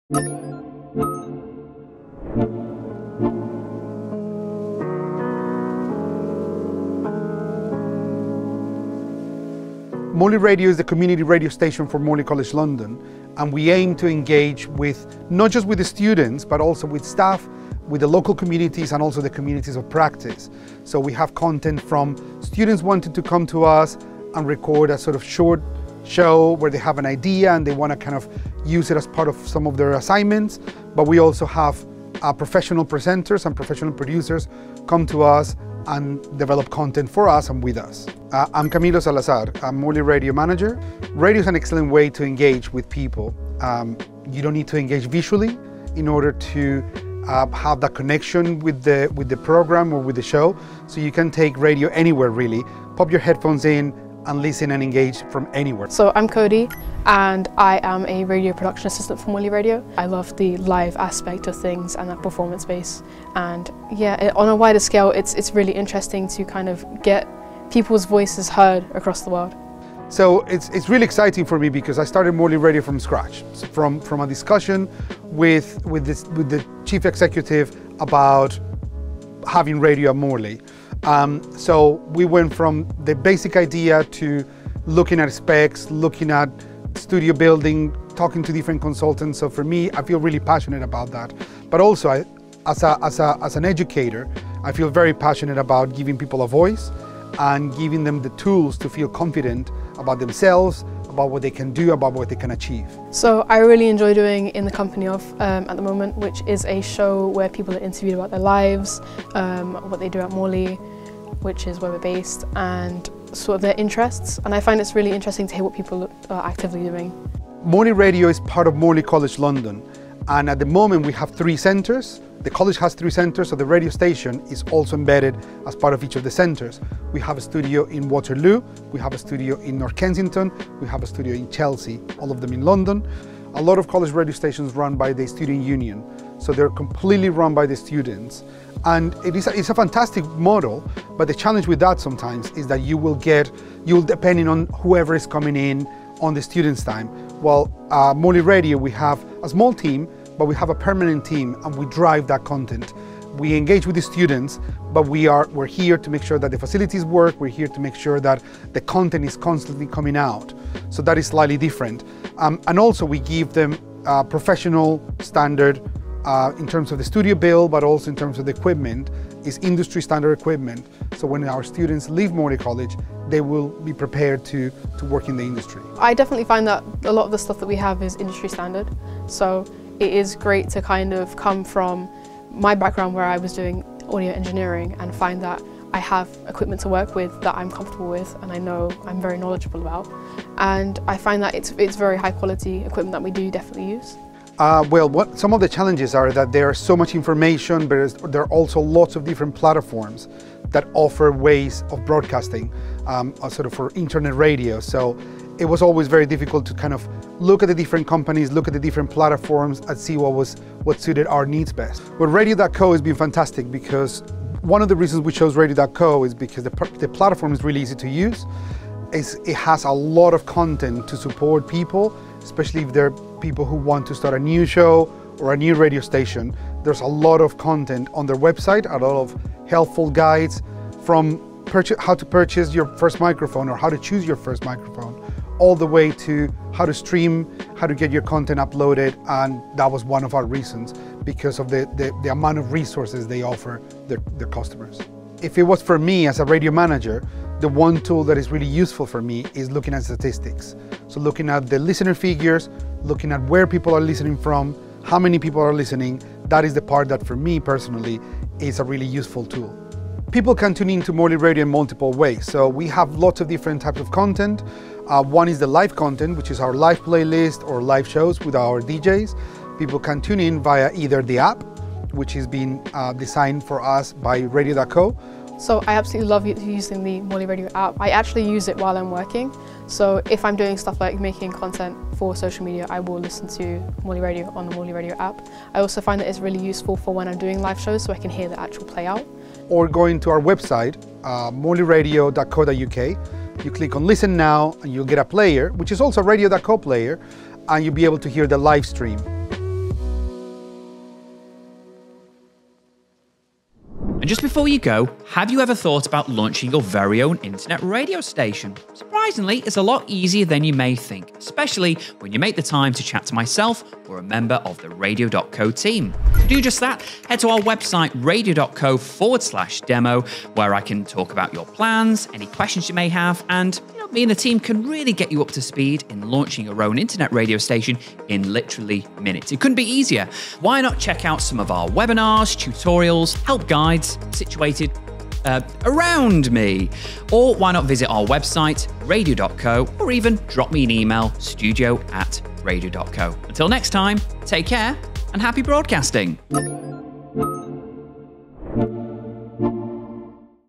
Allowing Students to Express Themselves Through Radio | Morley Radio Showcase
Morley Radio is a community radio station based in Morley College London, producing a wide range of radio programmes, podcasts and live output on a weekly basis. In today's video we see how Radio.co helps Morley Radio go live!